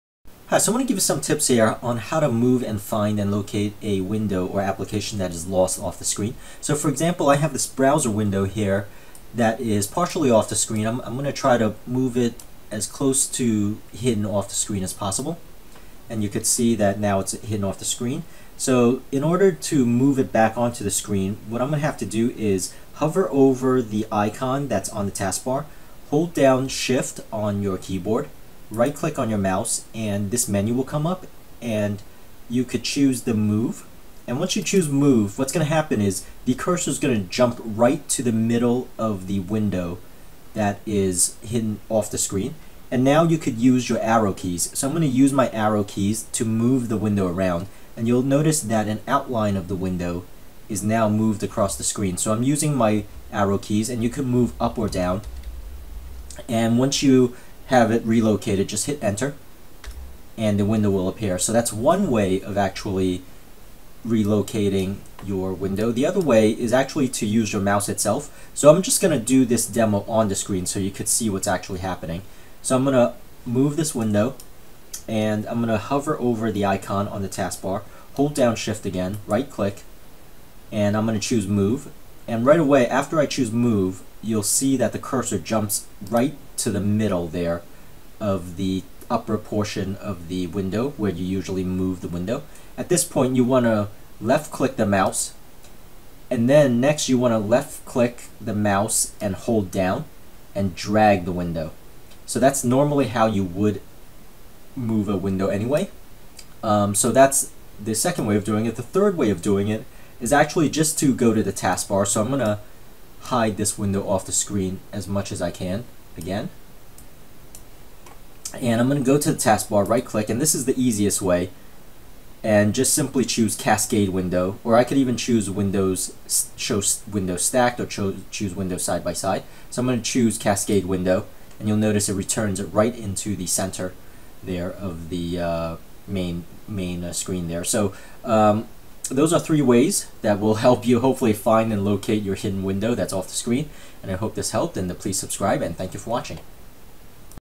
Find and Move Lost Window on Windows 11 and Windows 10
Where is your lost application window.  Sometimes you will lose your windows if you have multiple screens.  This will help you find and locate your lost window and relocated it to somewhere on the Desktop where you can interact with the Window.